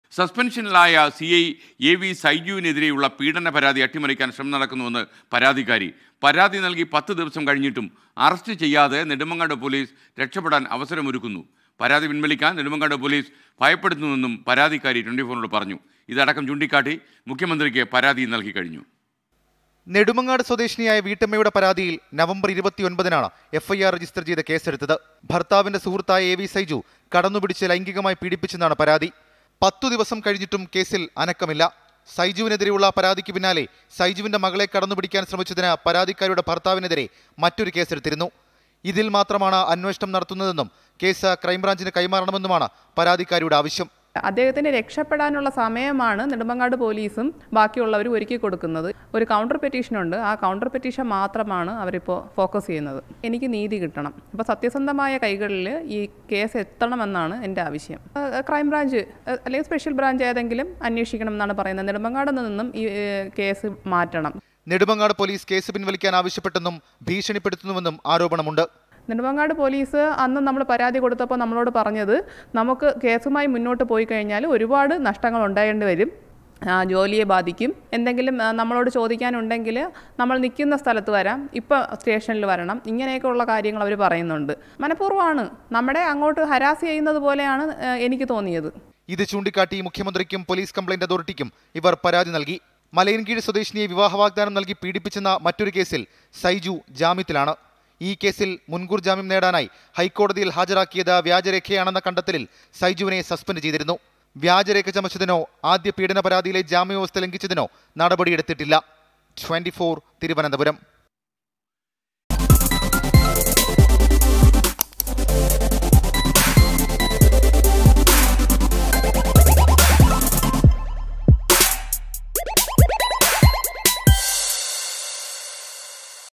എ.വി സൈജുവിനെതിരെയുള്ള പീഡന പരാതി അട്ടിമറിക്കാൻ ശ്രമം നടക്കുന്നുവെന്ന് പരാതിക്കാരി
സസ്പെൻഷനിലായ സി.ഐ എ.വി സൈജുവിനെതിരെയുള്ള പീഡന പരാതി അട്ടിമറിക്കാൻ ശ്രമം നടക്കുന്നുവെന്ന് പരാതിക്കാരി.പരാതി നൽകി 10 ദിവസമായിട്ടും അറസ്റ്റ് ചെയ്യാതെ നെടുമങ്ങാട് പോലീസ് രക്ഷപ്പടാൻ അവസരമൊരുക്കുന്നു.പരാതി പിൻവലിക്കാൻ നെടുമങ്ങാട് പോലീസ് ഭയപ്പെടുത്തുന്നുവെന്നും പരാതിക്കാരി 24നോട്